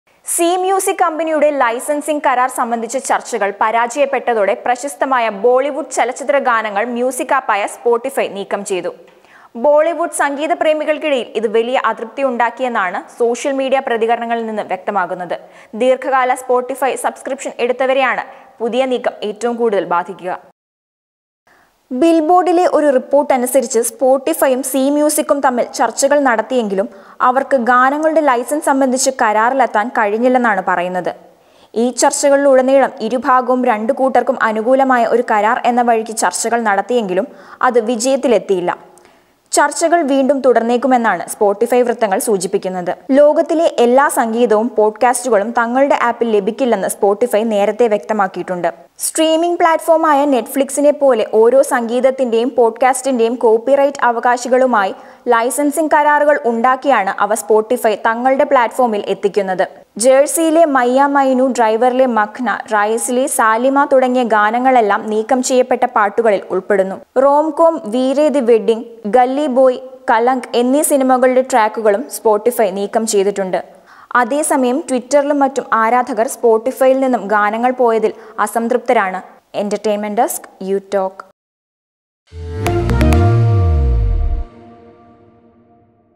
പ്രശസ്തമായ ബോളിവുഡ് ചലച്ചിത്ര ഗാനങ്ങള്‍ മ്യൂസിക്ക് ആപ്പായ സ്‌പോട്ടിഫൈ നീക്കം ചെയ്തു
സീ മ്യൂസിക് കമ്പനിയുടെ ലൈസന്‍സിംഗ് കരാര്‍ സംബന്ധിച്ച ചര്‍ച്ചകള്‍ പരാജയപ്പെട്ടതോടെ പ്രശസ്തമായ ബോളിവുഡ് ചലച്ചിത്ര ഗാനങ്ങള്‍  മ്യൂസിക്ക് ആപ്പായ സ്‌പോട്ടിഫൈ നീക്കം ചെയ്തു. ബോളിവുഡ് സംഗീത പ്രേമികള്‍ക്കിടയില്‍ ഇത് വലിയ അതൃപ്തിയുണ്ടാക്കിയെന്നാണ് സോഷ്യല്‍ മീഡിയ പ്രതികരണങ്ങളില്‍ നിന്നും വ്യക്തമാകുന്നത്. ദീര്‍ഘകാല സ്‌പോട്ടിഫൈ സബ്‌സ്‌ക്രിപ്ഷന്‍ എടുത്തവരെയാണ് പുതിയ നീക്കം ഏറ്റവും കൂടുതല്‍ ബാധിക്കുക.|youtalk|